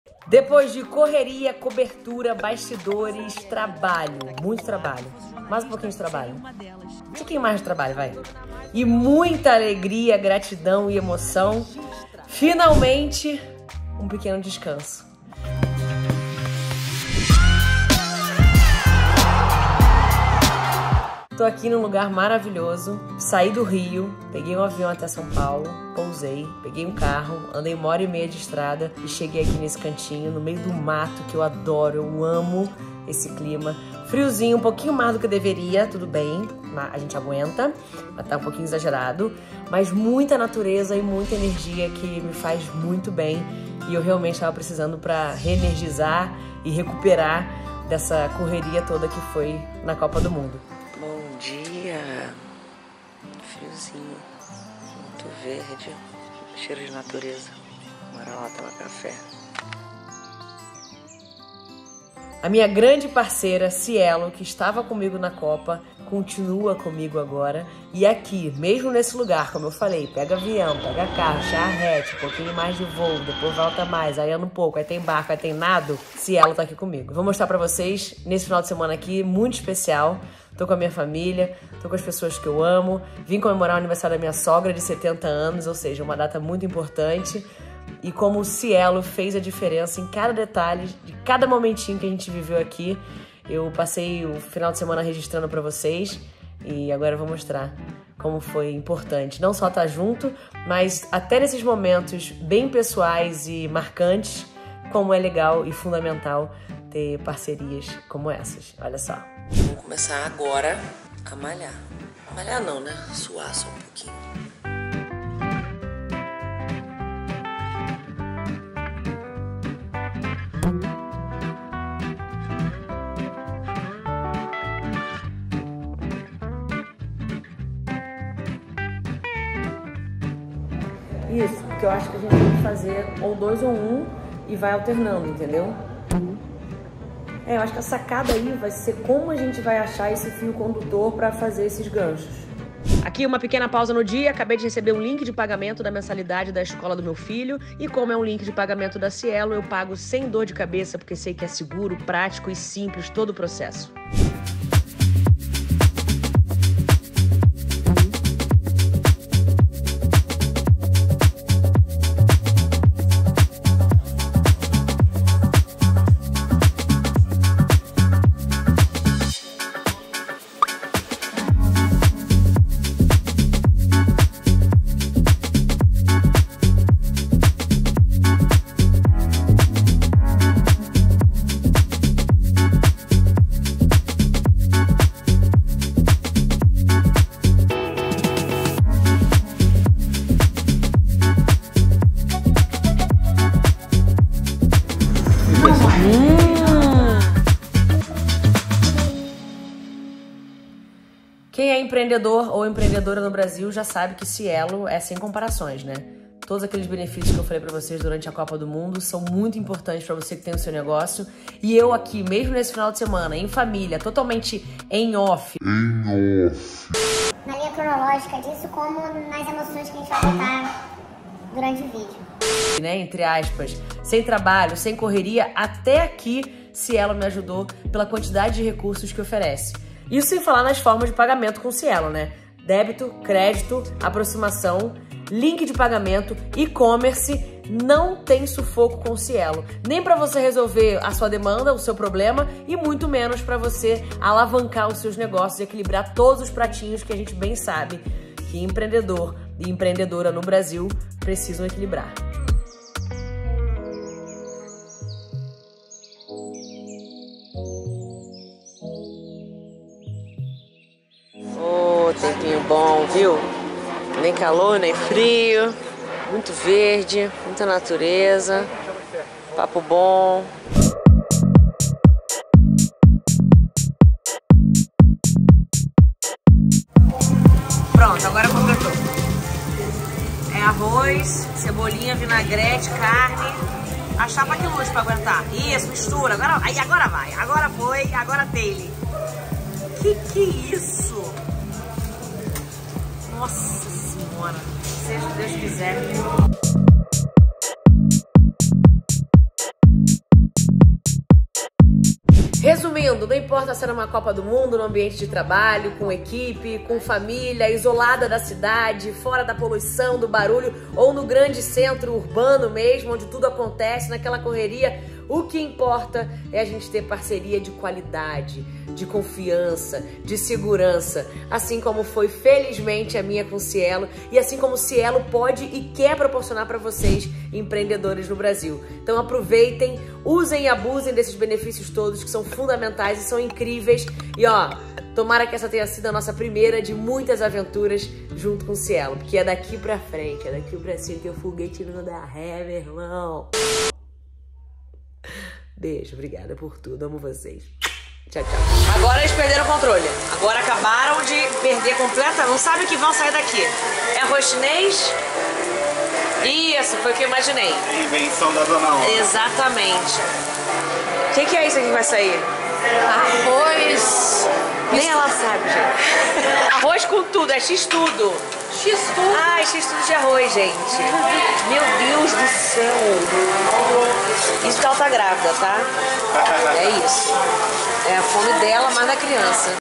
Passando TUDO no CRÉDITO mas alegrando a FAMÍLIA
Gentilmoreeees viajar é muito bom, mas voltar pra casa é melhor ainda! Ainda mais quando ainda rola uma fugidinha pra um ligar cheio de natureza, verde, friozinho e claro uma bela jaca pra gente enfiar o pé! Até porque com a minha parceirona @cielo eu não tenho stress NENHUM, só felicidade!!! AI QUE SAUDADE QUE EU TAVA. Enquanto vocês assistem o vídeo e me dizem qual cardápio é o preferido de vocês, eu vou continuar grudada aqui na minha família pra matar quem tava me matando!